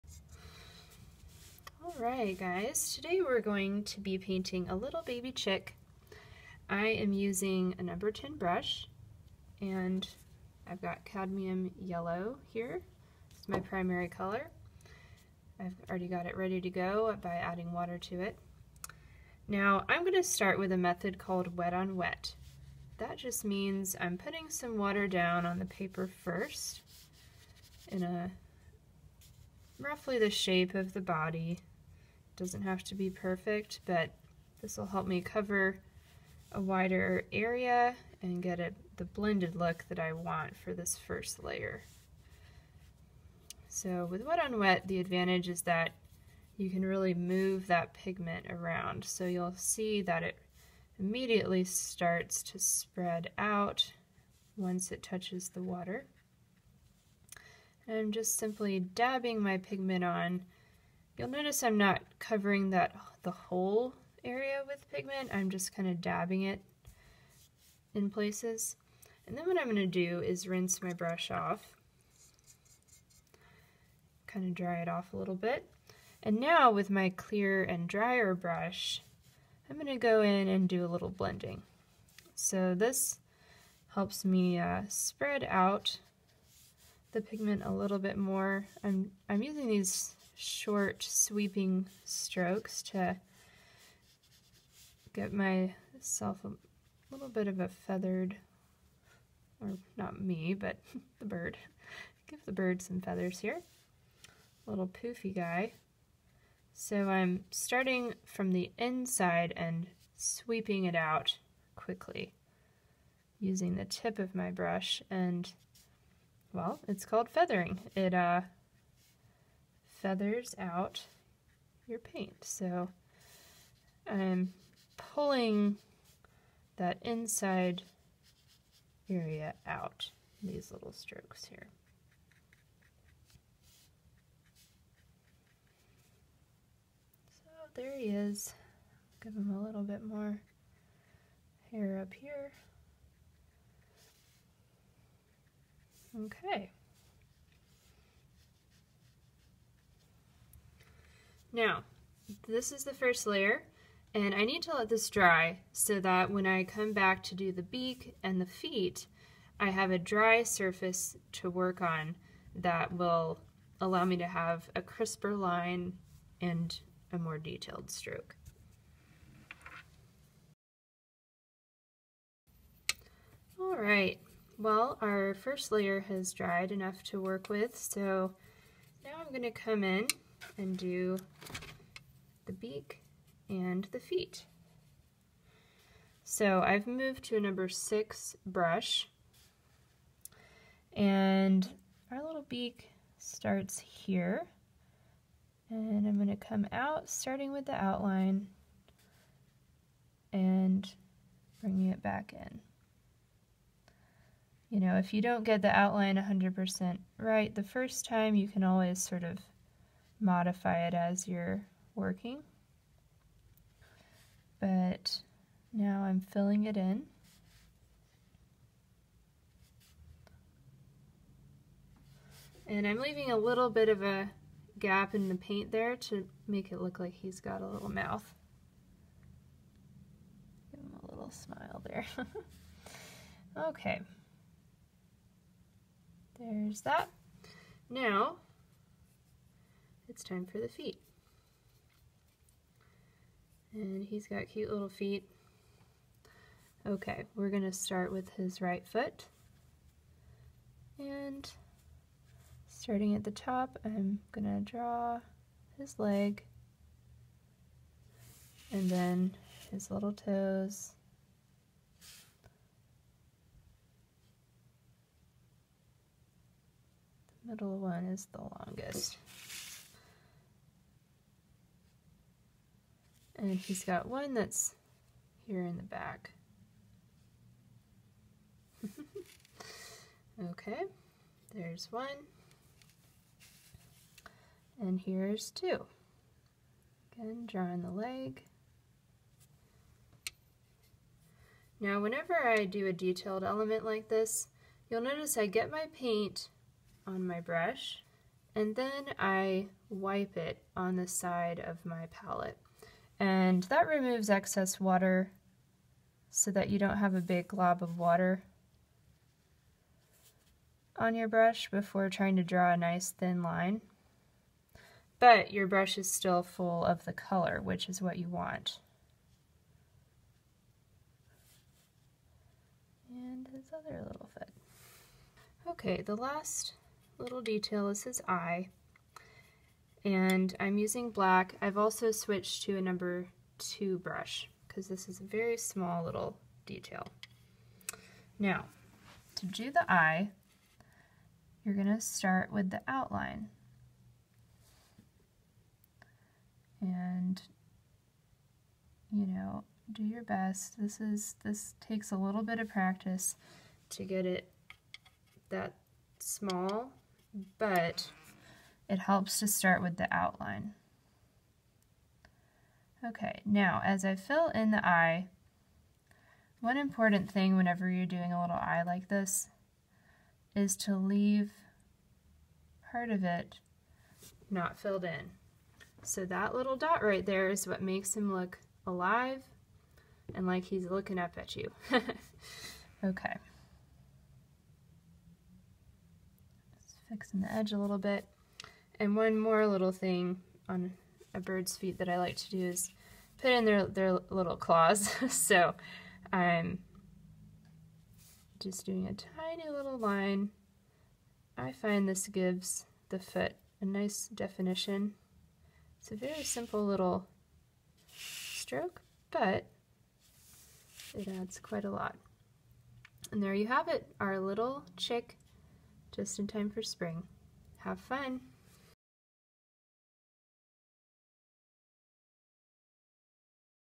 Paint a Spring Chick with Watercolor | Beginner Level Tutorial
This video will show you how to paint a simple spring chick using watercolors. I hope you'll join me as I walk you through these fun, easy steps!

The exact supplies I used are not critical, but I've listed everything below. Use whatever you can get your hands on! The main things to have available are some heavy paper, water, watercolor paint (either tube or pan), and a couple of brushes.

Supplies I Used:
- watercolor paper (#140 Cold Press)
- round watercolor brushes (sizes #10, #6, and #2 are used in the video)
- yellow paint (Holbein Cadmium Yellow)
- orange paint (Holbein Vermillion Hue)
- black paint (Holbein Ivory Black)

You can find more May We Fly watercolor tips and tutorials